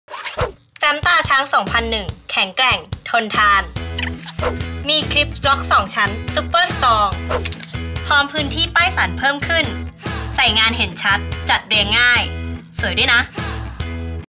สวยซุปเปอร์สตรอง...แฟ้มตราช้าง 2100
ไม่ว่าจุ๊จะโมโหอะไรมา ก็เอาอยู่!!!
สวยสตรองต้องแฟ้มตรงช้าง 2100 คลิปล็อค 2 ชั้น ซุปเปอร์สตรองงงงงงง
แข็งแรงทนทาน พร้อมพื้นที่ป้ายสันเพิ่มขึ้น ใส่งานเห็นชัด จัดเรียงง่าย พร้อมดีไซน์สวย